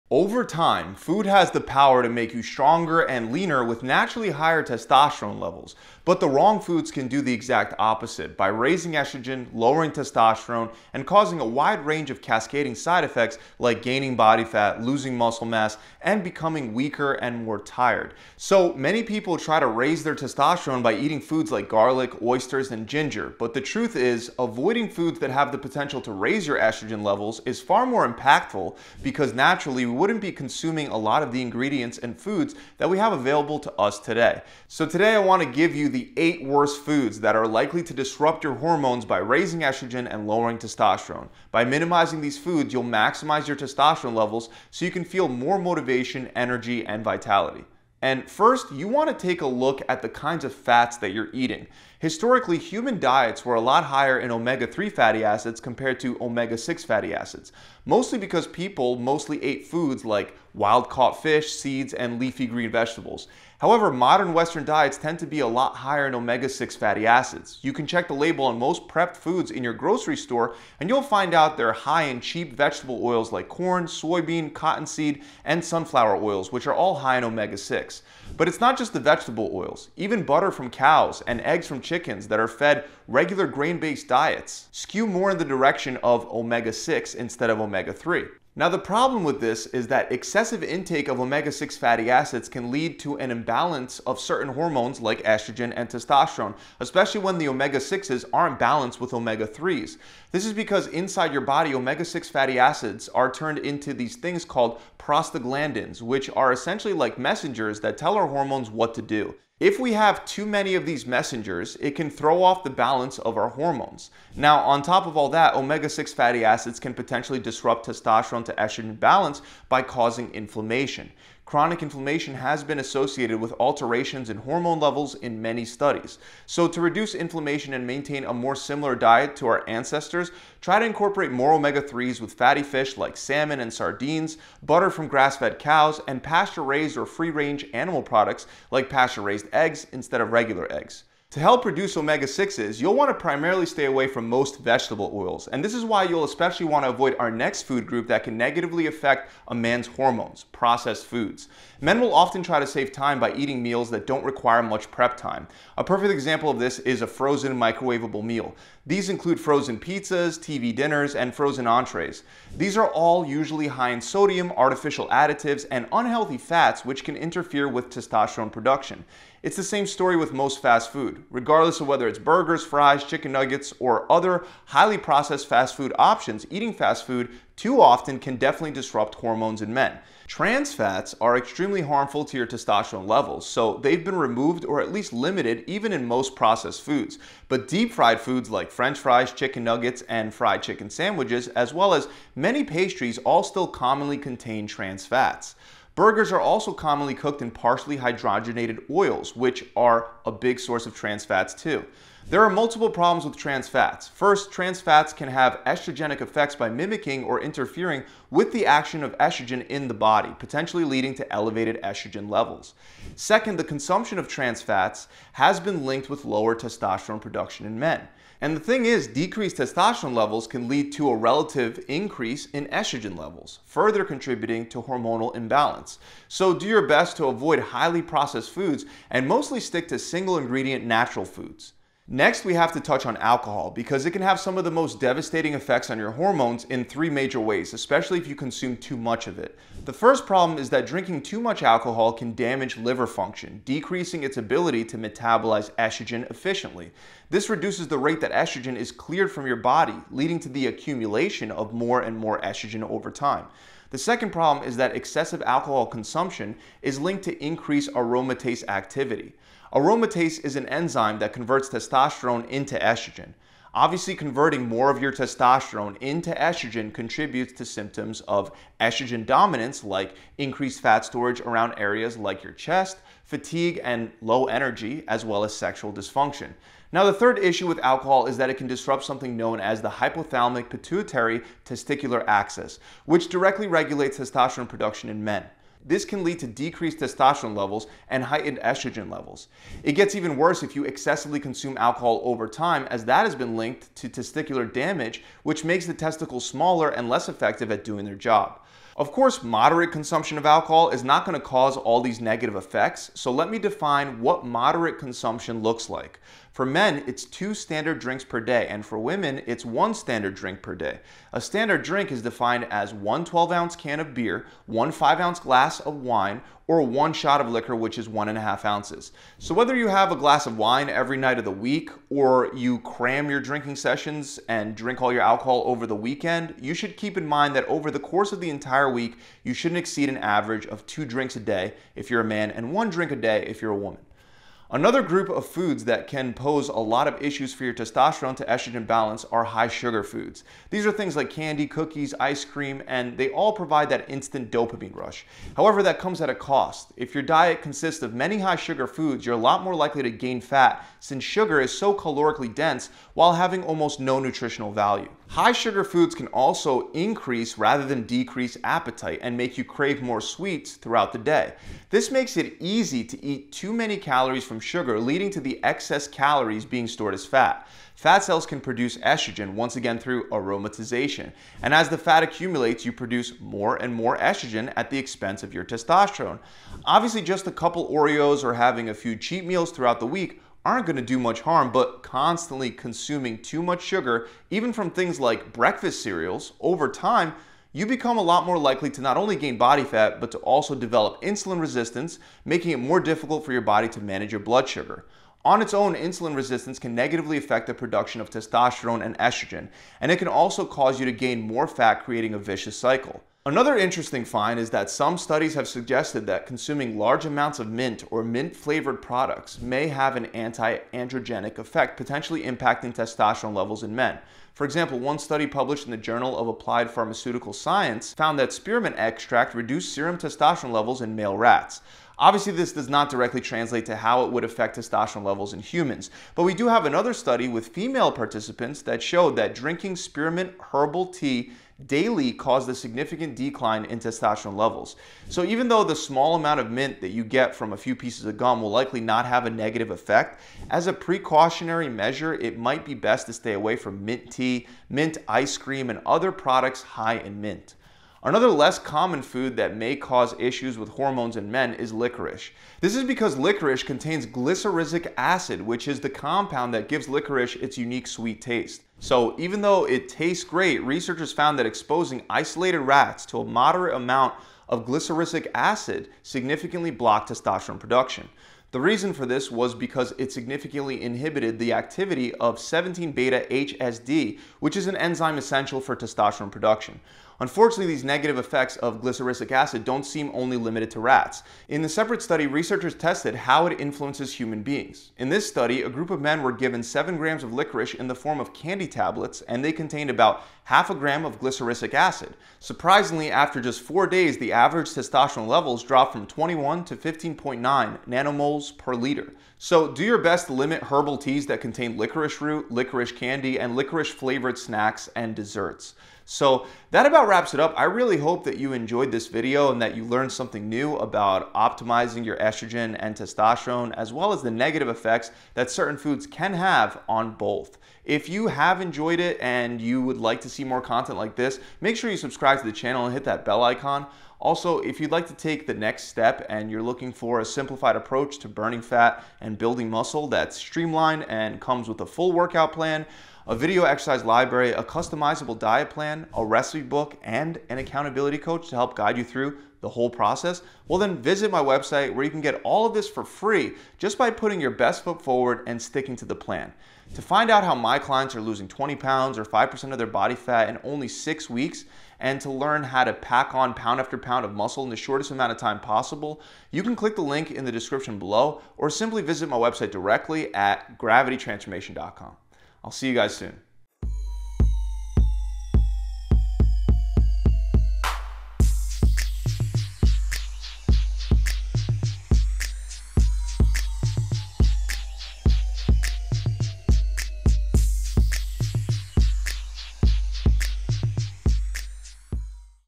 6 Testosterone Killers Men MUST Avoid
Boost your testosterone levels naturally by eliminating these high estrogen foods from your diet. If you want to have more drive and build more muscle and strength easier then rasing your testosterone levels will help. One of the best ways to do that is by reducing estrogen dominance through a proper diet.

Overtime food has the power to make you stronger and leaner with naturally higher testosterone levels but the wrong foods can do the opposite, by raising estrogen, lowering testosterone, and causing a wide range of cascading side effects like gaining body fat, losing muscle mass, and becoming weaker and more tired. So Many people try to raise their testosterone by eating foods like garlic, oysters, and ginger but the truth is avoiding foods that have the potential to raise your estrogen levels is far more impactful because naturally, we wouldn't be consuming a lot of the ingredients and foods that we have available to us today. So today I want to give you the 8 worst foods that are likely to disrupt your hormones by raising estrogen and lowering testosterone. By minimizing these foods you'll maximize your testosterone levels so you can feel more motivation, energy, and vitality.

And first you want to take a look at the kinds of fats that you're eating. Historically, human diets were a lot higher in omega-3 fatty acids compared to omega-6 fatty acids, mostly because people mostly ate foods like wild-caught fish, seeds, and leafy green vegetables. However, modern Western diets tend to be a lot higher in omega-6 fatty acids. You can check the label on most prepped foods in your grocery store and you'll find that they're high in cheap vegetable oils like corn, soybean, cottonseed, and sunflower oils which are all high in omega 6. But it's not just vegetable oils even butter from cows, and eggs from chickens that are fed grain-based diets skew more in the direction of omega 6. Now the problem with this is that excessive intake of omega-6 fatty acids can lead to an imbalance of certain hormones like estrogen and testosterone, especially when the omega 6's aren't balanced with omega-3's.

This is because inside your body Omega-6 fatty acids are turned into these things called prostaglandins, which are essentially like messengers that tell our hormones what to do. If we have too many of these messengers it can throw off the balance of our hormones. Now on top of all that, omega-6 fatty acids can potentially disrupt testosterone-to-estrogen balance by causing inflammation. Chronic inflammation has been associated with alterations in hormone levels in many studies. So to reduce inflammation and maintain a more similar diet to our ancestors try to incorporate more omega 3's with fatty fish like salmon and sardines, butter from grass-fed cows, and pasture-raised or free-range animal products like pasture-raised eggs instead of regular eggs.

To help reduce omega 6's you'll want to stay away from most vegetable oils and you'll especially want to avoid our next food group that can negatively affect a man's hormones, processed foods. Men will often try to save time by eating meals that don't require much prep time. A perfect example of this is a frozen, microwavable meal. These include frozen pizzas, TV dinners, and frozen entrees. These are all usually high in sodium, artificial additives, and unhealthy fats, which can interfere with testosterone production. It's the same story with most fast food. Regardless of whether it's burgers, fries, chicken nuggets, or other highly processed fast food options, eating fast food too often can definitely disrupt hormones in men. Trans fats are extremely harmful to your testosterone levels, so they've been removed or at least limited even in most processed foods. But deep-fried foods like French fries, chicken nuggets, and fried chicken sandwiches as well as many pastries all still commonly contain trans fats.

Burgers are also commonly cooked in partially hydrogenated oils which are a big source of trans fats too. There are multiple problems with trans fats. First trans fats can have estrogenic effects, by mimicking or interfering with the action of estrogen in the body, potentially leading to elevated estrogen levels. Second, the consumption of trans fats have also been linked to lower testosterone production in men. Decreased testosterone levels can lead to a relative increase in estrogen levels, further contributing to hormonal imbalance. So do your best to avoid highly processed foods and mostly stick to single ingredient natural foods.
Next, we have to touch on alcohol because it can have some of the most devastating effects on your hormones in 3 major ways especially if you consume too much of it. The first problem is that drinking too much alcohol can damage liver function, decreasing its ability to metabolize estrogen efficiently. This reduces the rate that estrogen